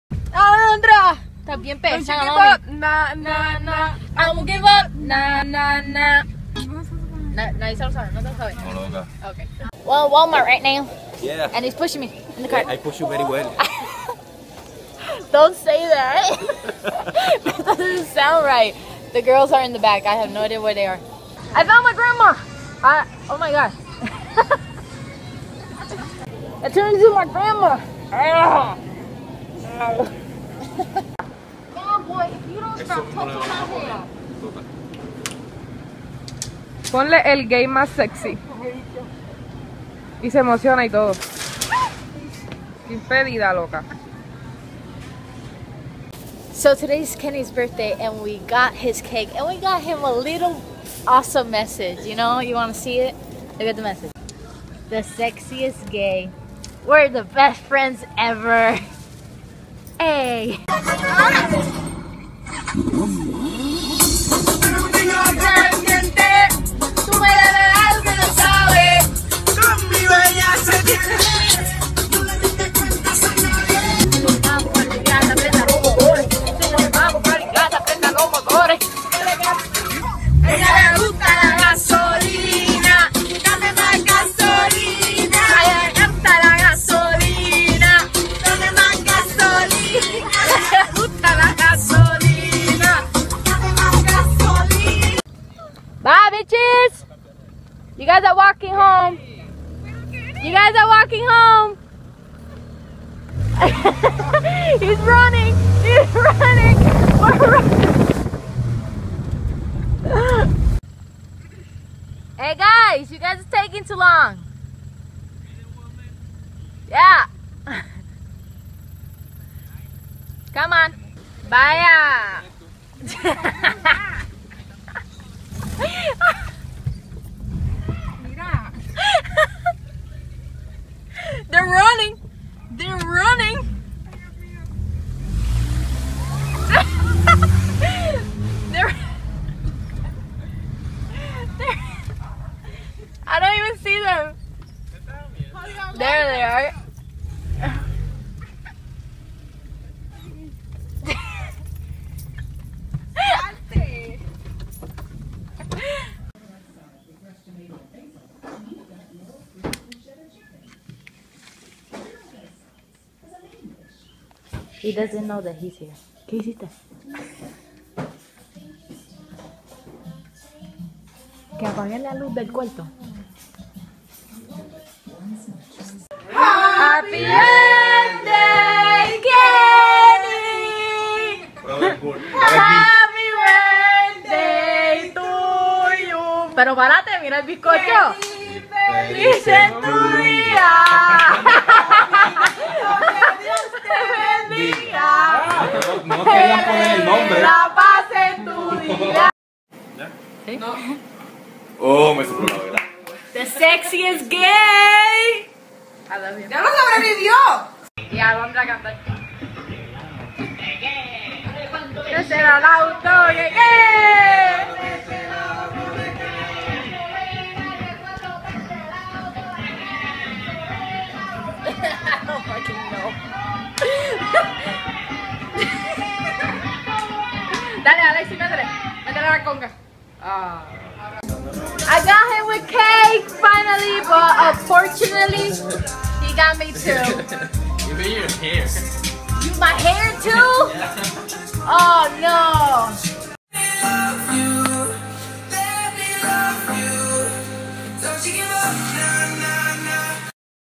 I FOUND MY GRANDMA
Celebrated my best friend Kenny's birthday. He turnned 20!!!
Usually don't post short vlogs like this one but I might be doing it more often.